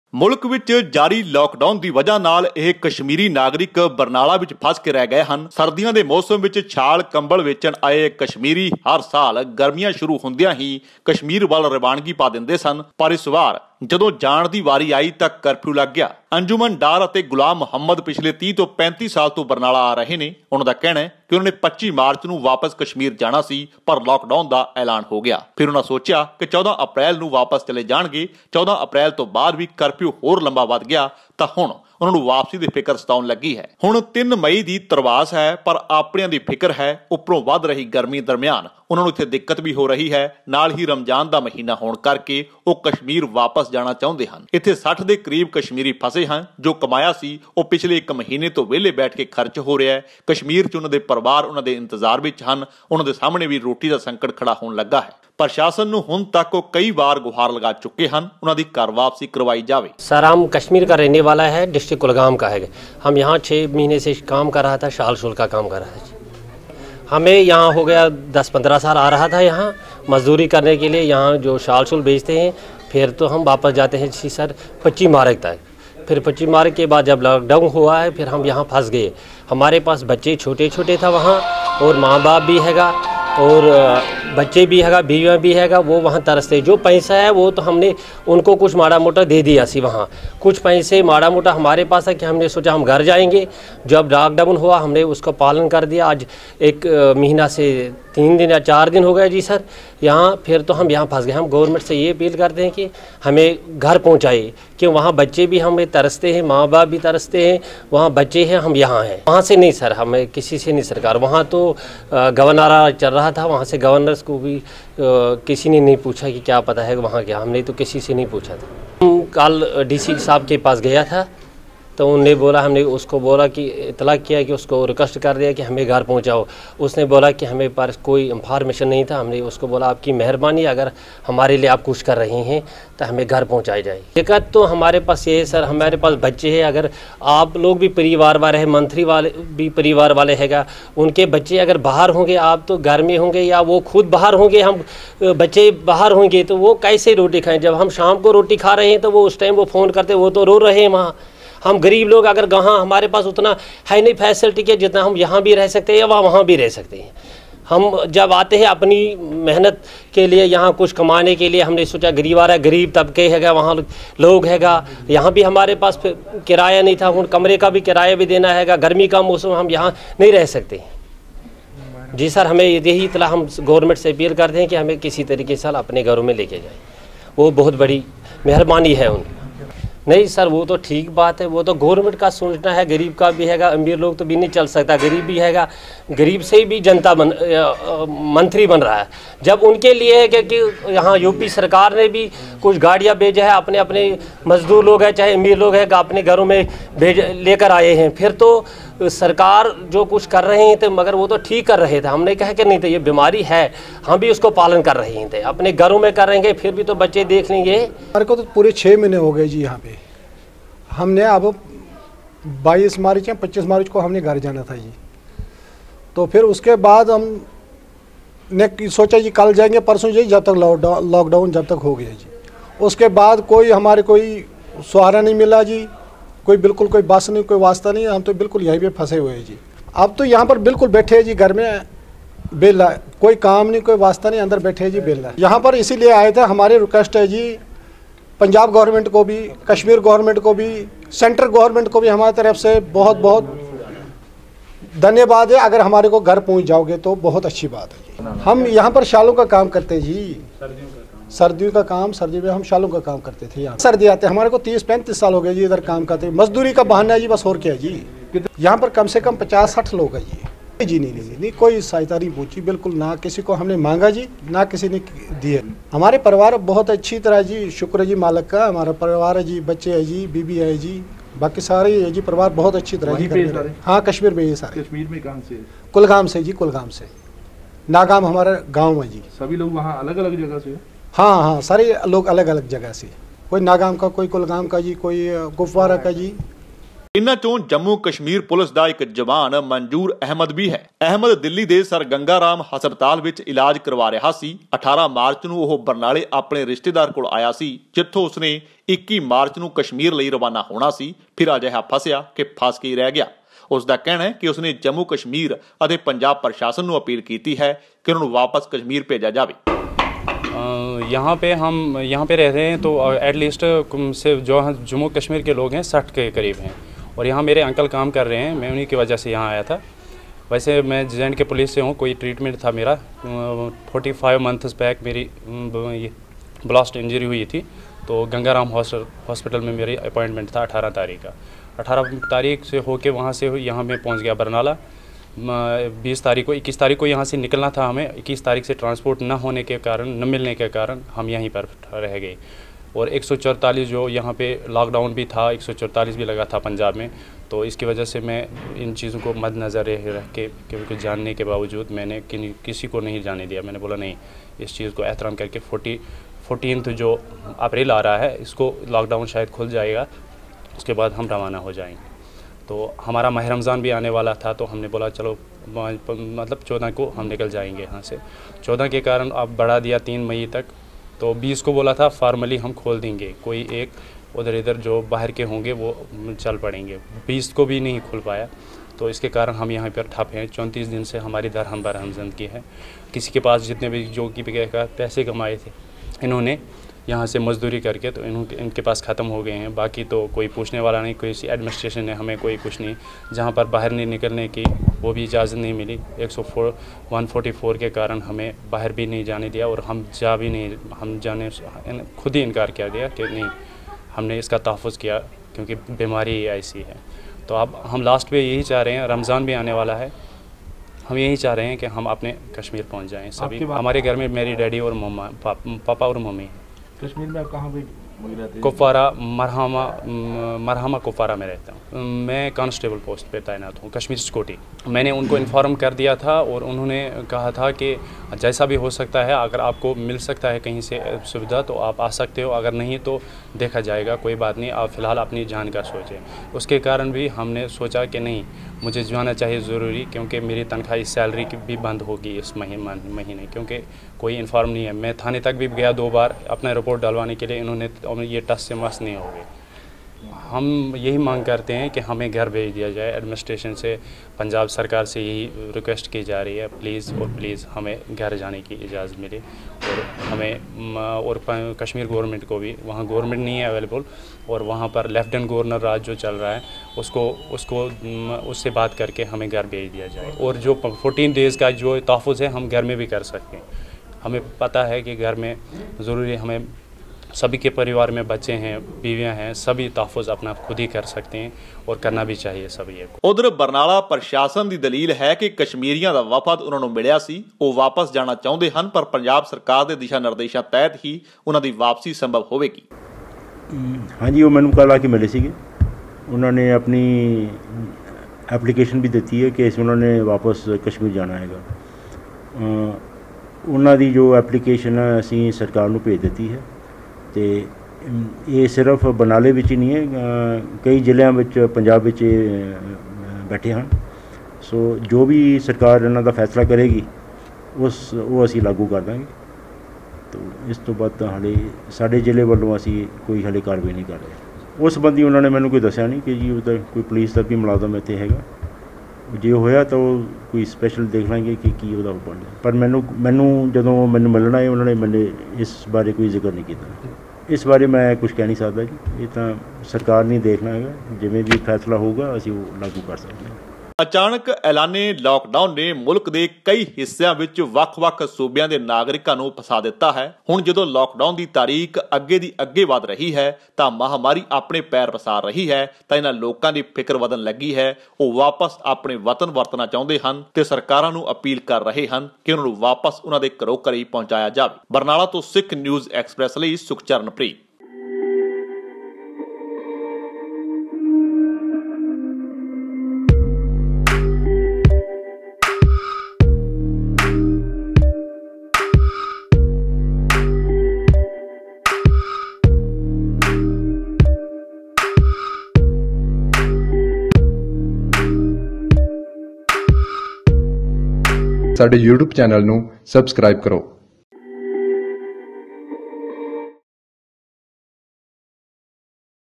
LOCK DOWN ਕਰਕੇ ਬਰਨਾਲਾ ‘ਚ ਫਸੇ 60 KASHMIRI ਪ੍ਰੇਸ਼ਾਨ, ਸਰਕਾਰਾਂ ਨੂੰ ਲਾਈ ਵਾਪਸੀ ਦੀ ਗੁਹਾਰ
LOCK DOWN ਕਰਕੇ ਬਰਨਾਲਾ ‘ਚ ਫਸੇ 60 KASHMIRI ਪ੍ਰੇਸ਼ਾਨ, ਸਰਕਾਰਾਂ ਨੂੰ ਲਾਈ ਵਾਪਸੀ ਦੀ ਗੁਹਾਰ || PUNJAB WIRE || SNE